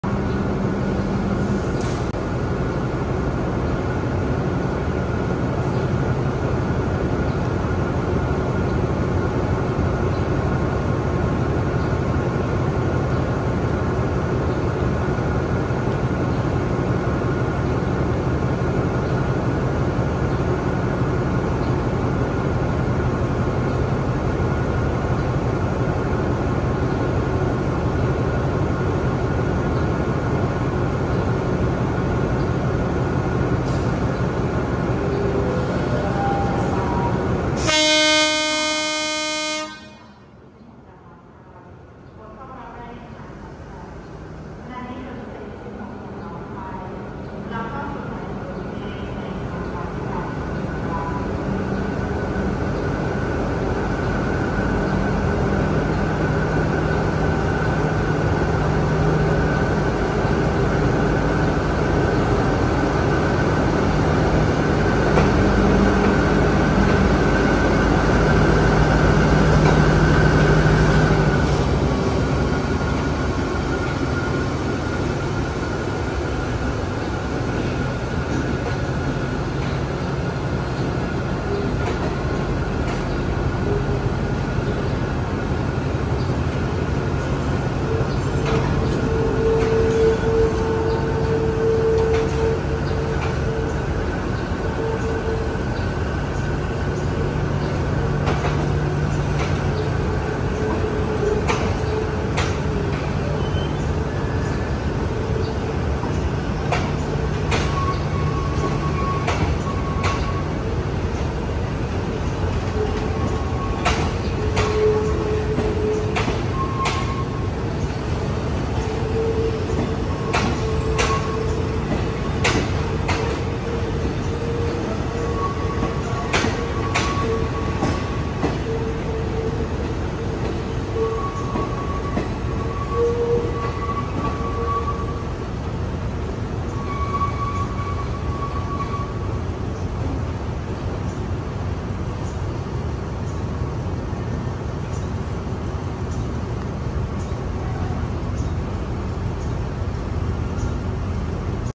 Rotfaithai Ep.1132 อัลสตอม 4152 ทำขบวนรถธรรมดา 275 บ้านคลองลึก ออกจากชาน 6 หัวลำโพง
Train : Ordinary 275 Bangkok - Ban Klong Luek Border
Locomotive : Alsthom 4152
Date time : February 6, 2022, 5.54 AM.
Place : Bangkok Station

ขบวนรถ : รถธรรมดา 275 กรุงเทพ - ด่านพรมแดนบ้านคลองลึก
รถจักร : อัลสตอม 4152
วัน-เวลา : 6 กุมภาพันธ์ 2565, 5.54 น.
สถานที่ : สถานีกรุงเทพ